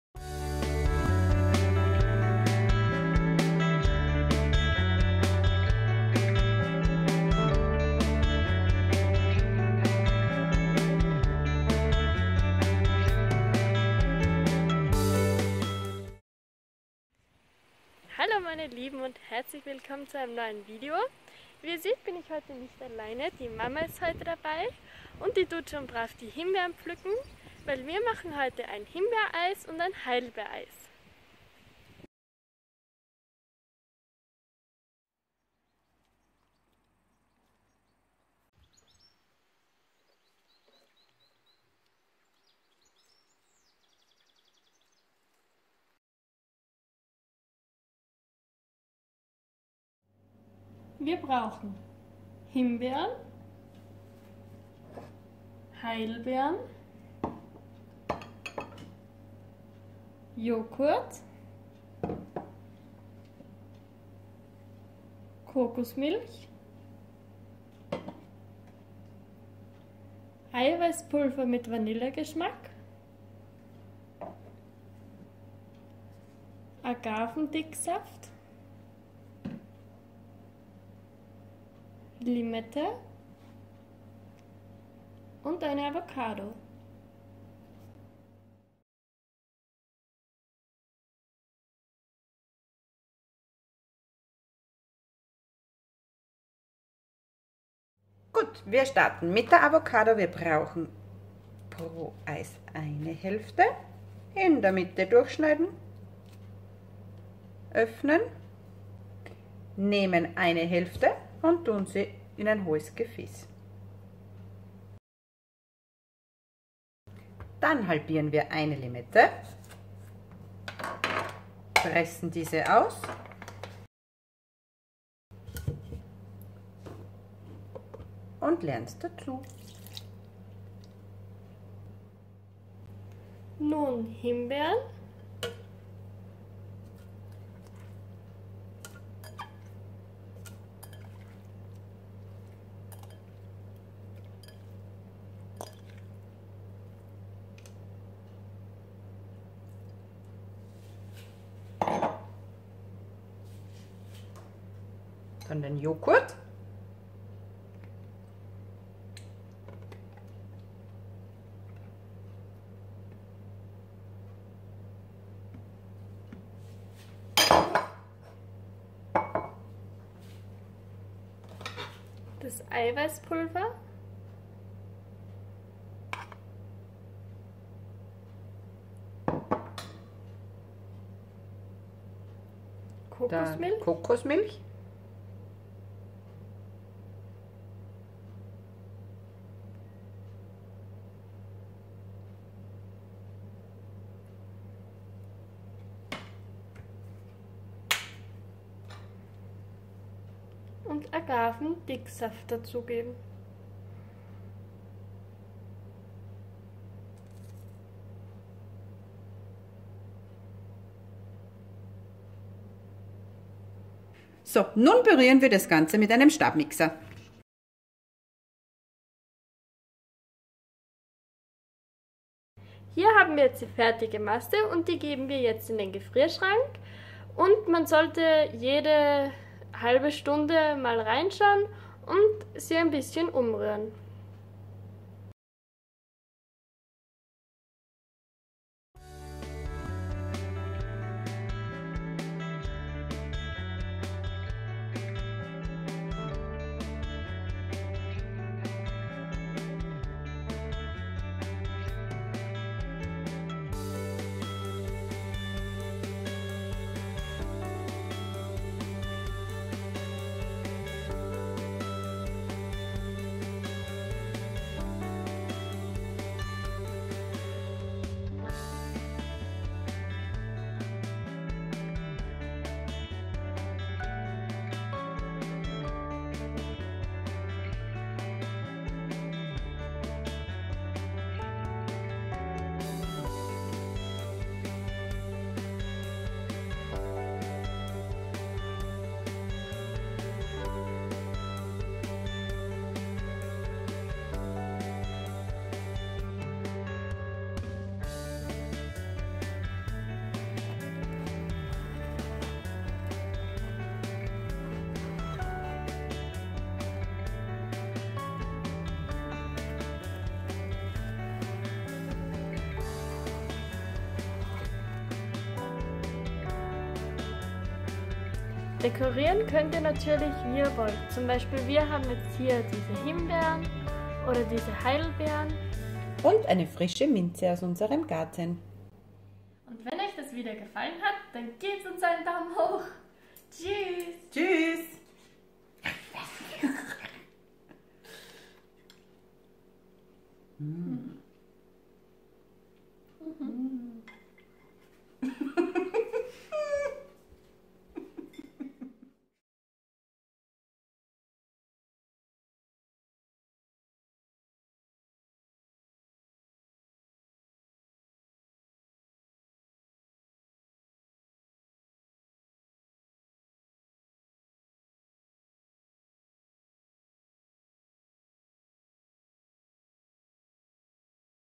gesunde Snacks | selbstgemachtes Himbeer & Heidelbeer Eis - ohne Eismaschine - ohne Zucker
WILLKOMMEN bei den gesunden Snacks: heute zeigen wir Euch, wie man schnelles, einfaches leckeres und gesundes Himbeer- & Heidelbeereis selber macht! (ohne Eismaschine!)

🛒 Ihr braucht dazu: 🛒
✔ frische Himbeeren ca. 300g
✔ 100 ml Kokosmilch
✔ 250g Naturjoghurt cremig
✔ 2 EL Eiweißpulver mit Vanillegeschmack
✔ ½ Avocado
✔ Saft einer ½ Limette
✔ Agavendicksaft (1 – 2 EL)

Für das Heidelbeereis nochmals die gleiche Menge allerdings statt Himbeeren, ca. 300g Heidelbeeren!

Und so geht’s:
Die Avocado halbieren und mit einem Löffel das Fruchtfleisch aus der Schale lösen. Alle Zutaten in einen hohen Rührbecher geben und mit einem Pürierstab zu einer cremigen Masse mixen.
Die cremige Fruchtmasse in den Gefrierschrank geben, und zwischendurch immer wieder gut durchrühren! Es wird ein paar Stunden dauern bis das Eis fest wird, am Besten schon am Vortag zubereiten!
💡 WICHTIG: ca. alle 30 Minuten die Masse aus dem Gefrierschrank nehmen, gut mit einer Gabel umrühren, und wieder in den Gefrierschrank stellen. Je öfter man die Eismasse umrührt desto kleiner bleiben die sog. „Eiskristalle“ und das Eis wird cremiger, weiters können sich die Zutaten NICHT voneinander „trennen“ (Wasser & Fett) diesen Vorgang ca. 4 – 5 x wiederholen
💡 TIPP: Sollten sich Kristalle bilden, dann einfach nochmal kurz mit dem Pürierstab mixen!💡

Hier verwenden wir statt der sehr fetthaltigen Sahne (ungesunde Fette) – Kokosmilch und Avocado.  Die Avocado ist reich ein essentiellen, ungesättigten Fettsäuren und daher sehr gesund. Die Süße kommt aus den Früchten bzw. vom Agavendicksaft.
Himbeeren sind reich an Vitamin C, A, B, Kalium, Kalzium, Folsäure
Heidelbeeren sind reich an Vitamin C, E, Folsäure, Kalium
Beeren zählen zu den heimischen „Superfoods“ sind reich an Antioxidantien und stärken unser Immunsystem.

💡 TIPP: Natürlich könnt Ihr nach Lust und Laune variieren! Statt Himbeeren / Heidelbeeren kannst Du gerne andere Früchte zB Mango, Erdbeeren,… verwenden, statt Kokosmilch nehmt einfach mal Mandelmilch, Sojamilch…  Für mehr Süße gebt 2 EL Agavendicksaft oder Honig hinein.
Statt Naturjoghurt nehmt Kokosmilch-Joghurt, Soja-Joghurt - statt Eiweißpulver verwendet Sojaeiweißpulver (vegane Variante) ….

weil’s g’sund ist und schmeckt – nur du bestimmst was reinkommt!

Viel Spaß beim Nachmachen und gutes Gelingen!!!
Lisa und Jenny

Dieses Video erfolgt in keiner Kooperation und alle Meinungen sind unsere eigenen 😊. Auch die Produkte haben wir selbst erworben.